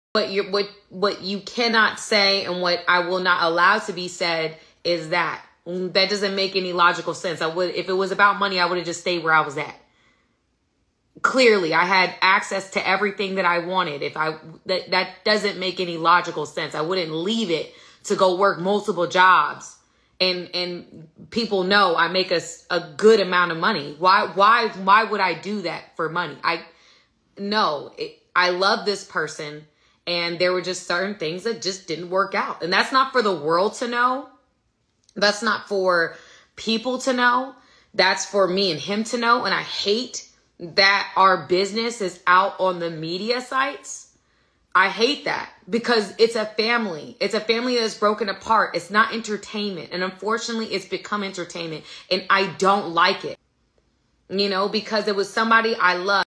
Tyrese ex-wife wife Samantha Lee speaks out after tyrese told the internet that their relationship w
Tyrese ex-wife wife Samantha Lee speaks out after tyrese told the internet that their relationship was based off money and status